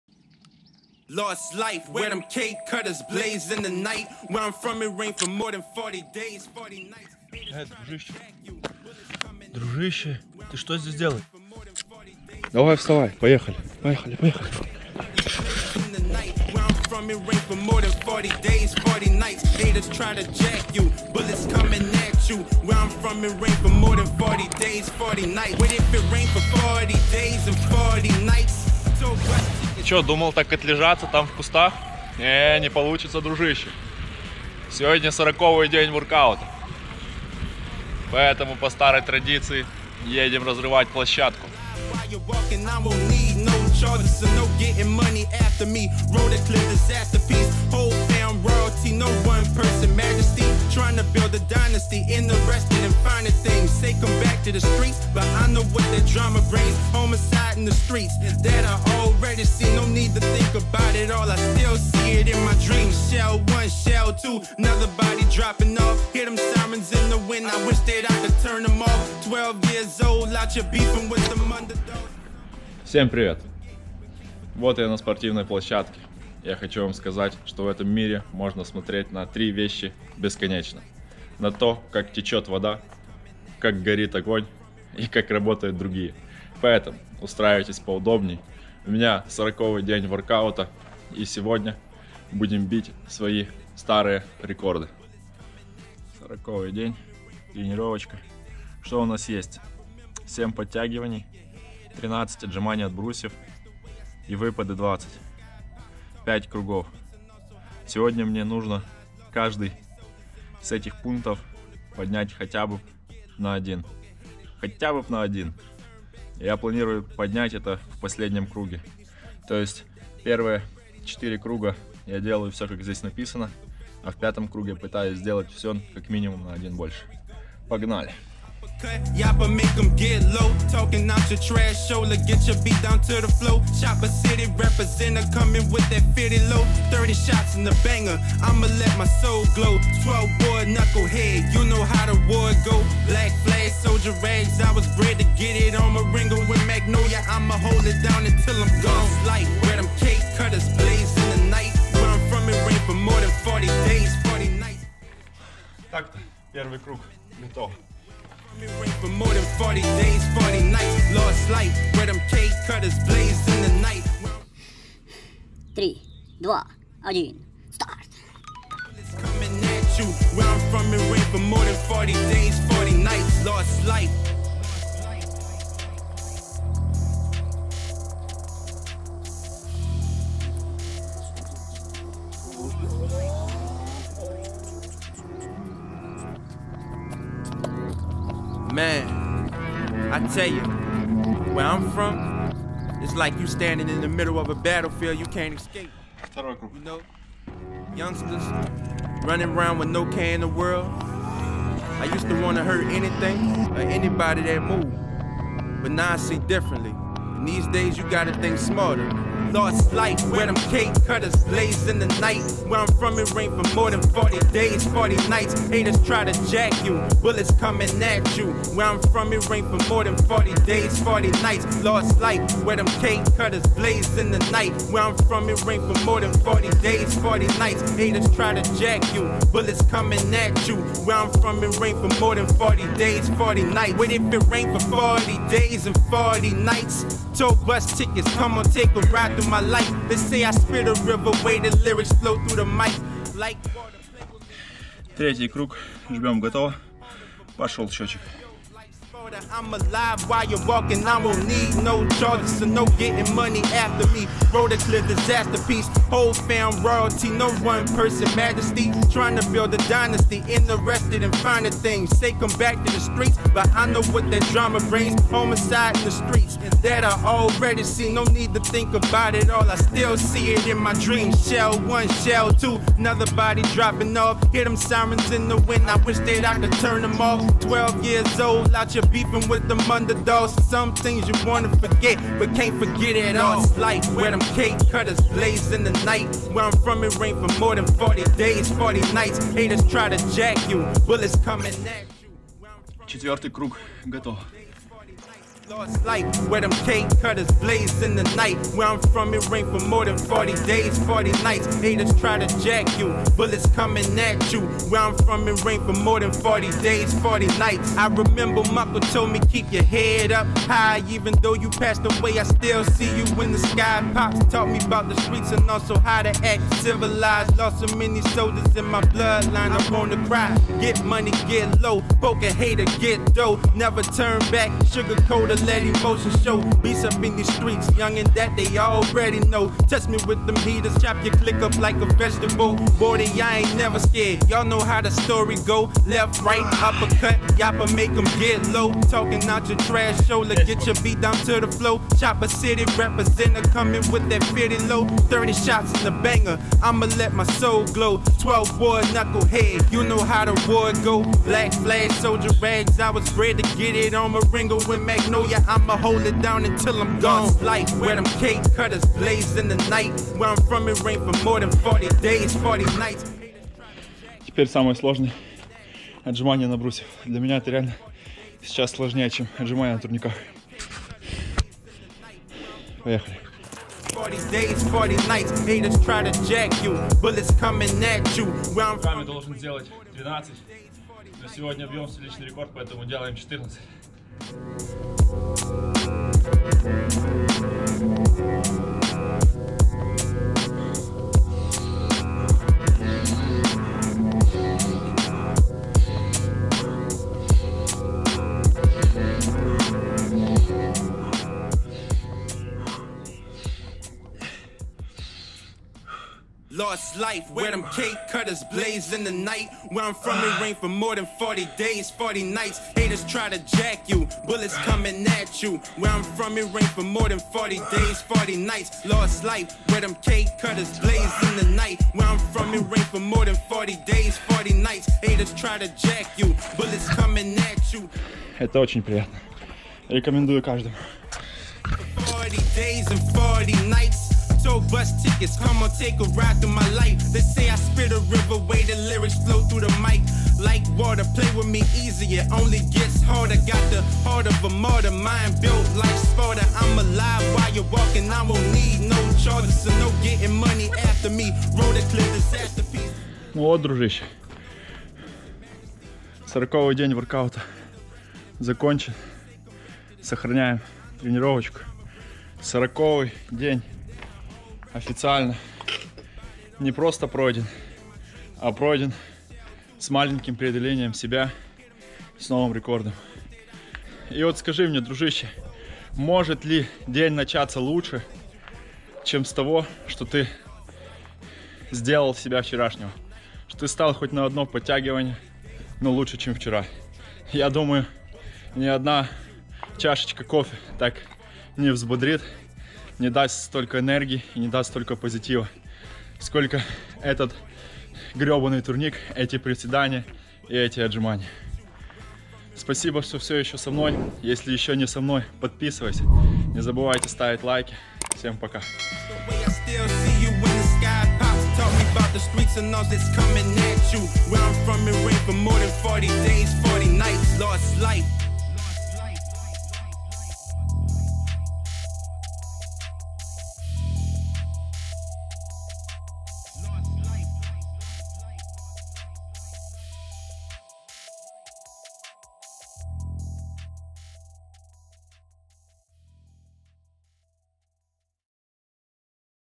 Влог 11. 40 дней пройдены!
▰Привет!) Рад тебя видеть снова) у меня уже 40-ковый день позади, есть прогресс, есть результат - и это очень круто! Уже жду 50-тый день, первый чекпоинт на котором все мы увидим к чему меня приведут эти 50 дней))) пс. также я был в центре, закупил все что нужно было для мини студии, также есть уже материалы с тренировки возле моря) как говорится "coming soon".)Ребята будьте умными и не материтесь в комментариях :D
▰Игорь Кузьмин▰